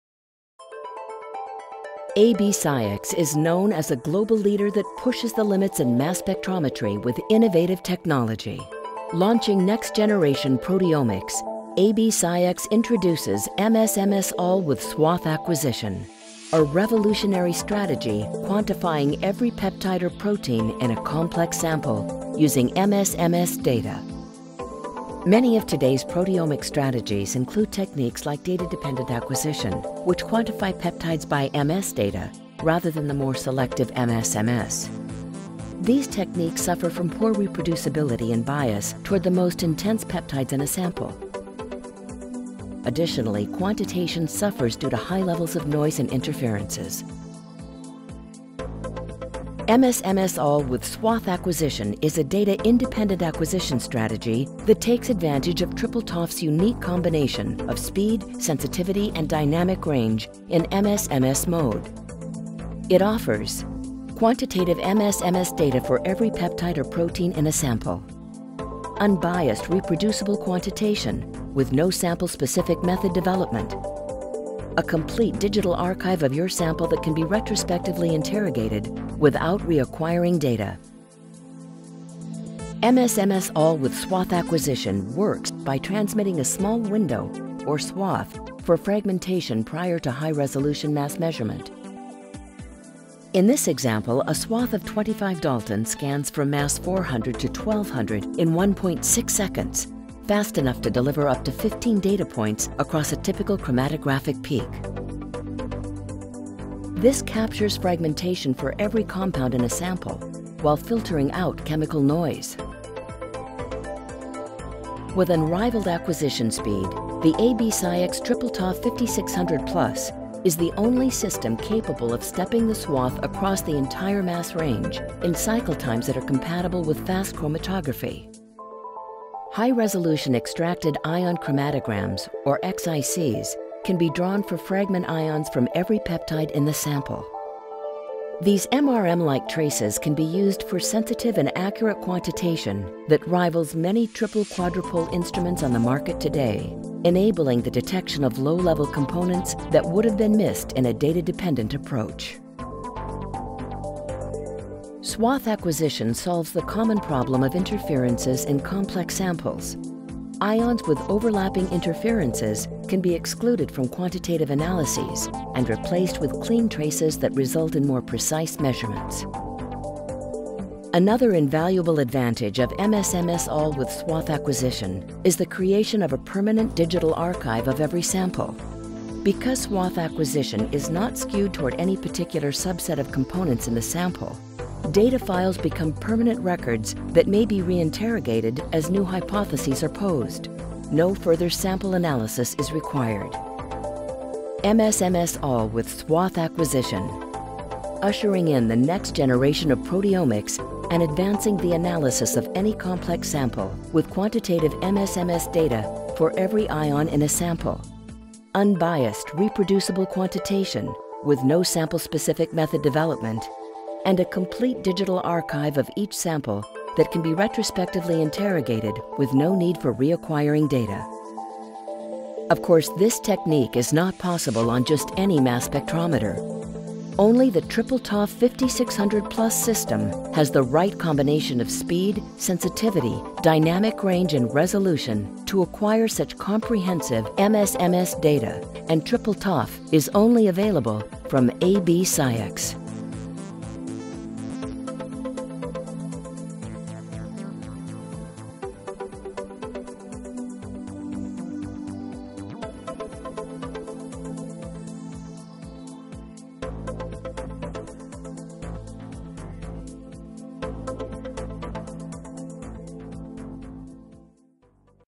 MS/MS ALL with SWATH Acquisition
MS/MSALL with SWATHTM Acquisition is a novel data-independent acquisition technique that generates a complete digital archive of all possible ions in a single analysis for permanent, retrospective, quantitative MS/MS analysis of complex samples.  Accomplished by systematically fragmenting all ions on an LC-compatible time scale, this technique is only available on the AB SCIEX TripleTOF® 5600 and 5600+ Systems, which provide the speed, sensitivity, and dynamic range required to deliver the highest quality results.